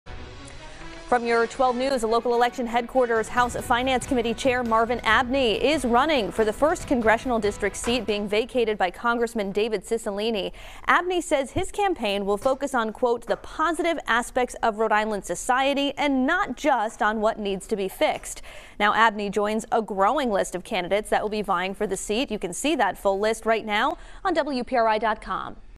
Marvin Abney joins fray to replace Cicilline for Congress; 12th Dem to join
R.I. House Finance Committee Chairman Marvin Abney has announced he's tossing his hat into the ring for Rhode Island's 1st Congressional District, joining a rapidly thickening throng of Democrats seeking to replace outgoing Congressman David Cicilline.